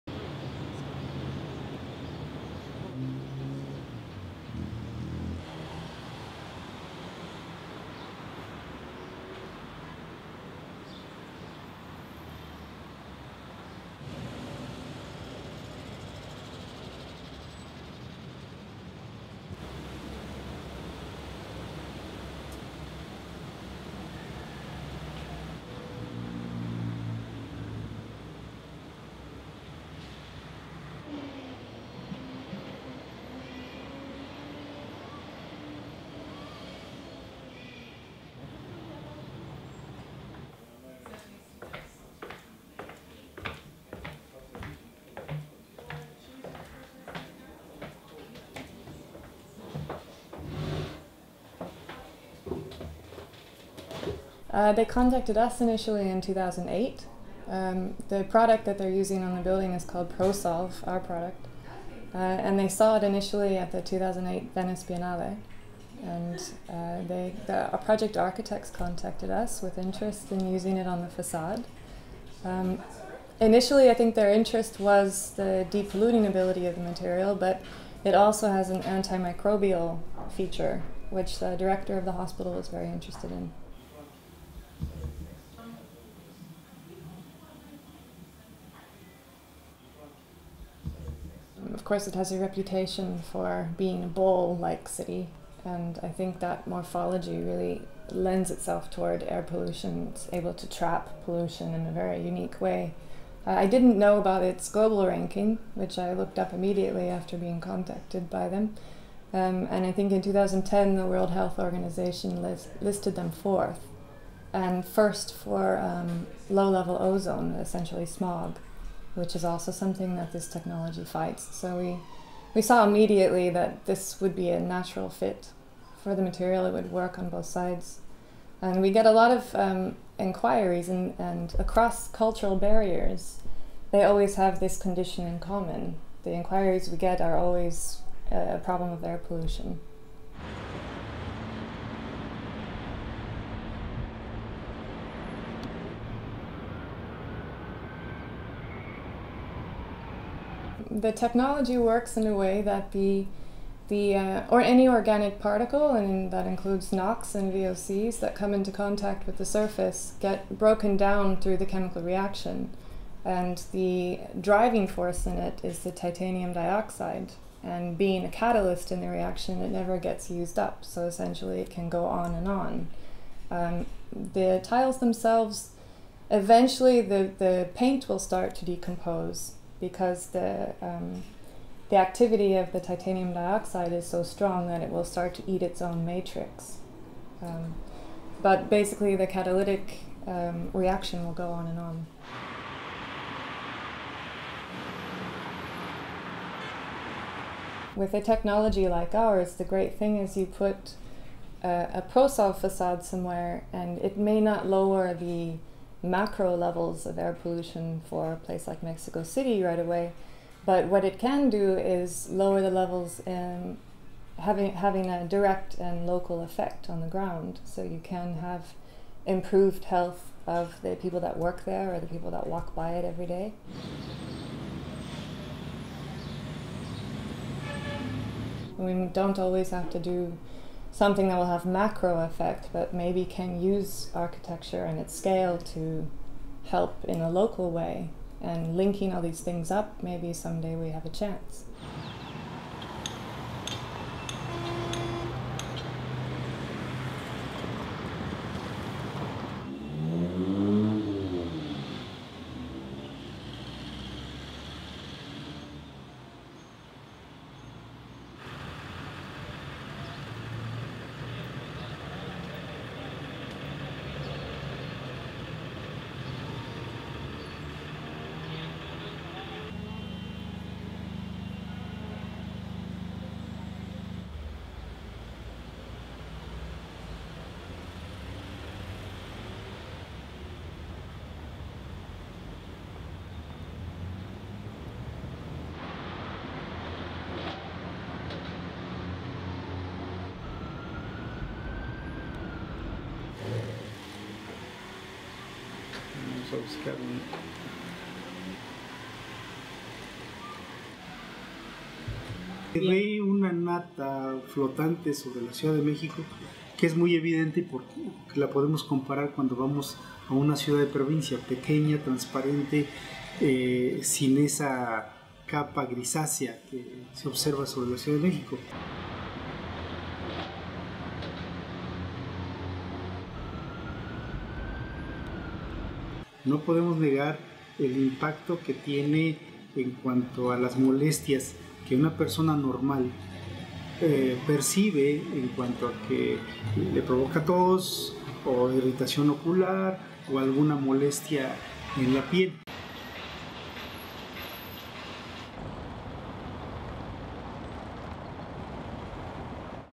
Hospital built with smog eating cladding
(27 Sep 2013) In the centre of one of the world's most polluted cities a new hospital is being built from high tech materials which purify the air around it.
It's thought to be the first public building to use such smog-eating technology.
STORYLINE
Beneath the heavily polluted sky this building is at the vanguard of architecture in Mexico City.
Surrounded by fumes and pollution the cladding has a 24 hour fight to purify the air.
The hospital is based in the southern part of the city, and is still partly covered by scaffold as builders carry out the finishing touches.
The new wing was set to open in November this year (2013), but the opening has been postponed.
Nearly three thousand square feet (2500 square metres) of the building is covered in what appears to be an ornate sculpture.
But this is the essential part of the structure designed by architectural firm  Elegant Embellishments, which is hoping to provide an alternative solution to urban pollution.
Partner Allison Dring says: "They contacted us initially, in 2008, the product we are using in the building is Prosolve, our product, and they saw it initially at the 2008 Venice Biennale and the project architects contacted us, with interest in using it on the facade. Initially their interest I think it was in de-polluting ability of the material but it also has an anti-microbial feature which the director of the hospital was very interested in."
The white structure is a smog-eater which neutralises emissions and other toxins, cleaning the air as it enterers the building it surrounds.
It's composed of a material called Prosolve370e and it's activated by ambient daylight.
The building is made of modules of the tiles coated with titanium dioxide, which claims  to have self-cleaning and anti-microbial qualities.
Dring explains how Prosolve works: "The technology works in a way that the, or any organic particle that includes Nox (nitrous oxides), and VOCS (volatile organic compounds) that come into contact with the surface get broken down through the chemical reaction and the driving force in it is the titanium dioxide and being a catalyst in the reaction it never gets used up so essentially it can go and on. The tiles themselves eventually the paint will start to decompose because the activity of the titanium dioxide is so strong that it will start to eat its own matrix but basically the catalytic reaction will go on and on. "
The company has already used Prosolve370e in small structures placed inside a shopping-centre in Perth, Australia and is currently working on a project inside a hotel in Abu Dhabi.
The Mexico project is the largest to date and also the first one designed for the exterior of a building.
An added bonus is that the Prosolve 370e acts as a natural light filtration system and solar gain blocker for the interior of hospital, effectively reducing the heat and electricity bill.
In 1992 Mexico City was declared by the United Nations to the most polluted city in the world, a doubtful honour that prompted the authorities to take urgent measures.
Petrol had to be reformulated so that it was less polluting, private cars were banned from the city one day a week, factories were moved, or shut down and vehicles produced for the Mexican market were required by law to have catalytic converters.
The virtually non existent public transportation system was also improved.
Nowadays the 20 million-strong capital is out of the top ten of the world's most polluted cities, but the number of cars has doubled, suburbs keep growing and some residents continue to break anti pollution laws.